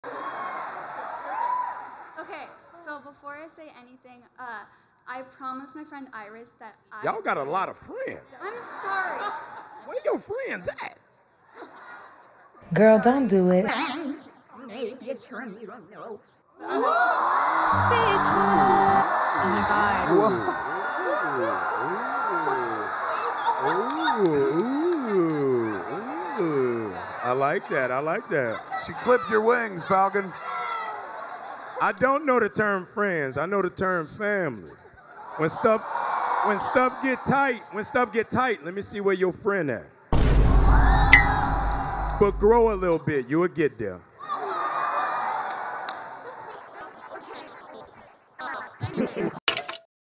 Tom Holland fan tries to roast Anthony Mackie 👀
Tom Holland fan tries to roast Anthony (it was a bad idea... 💀)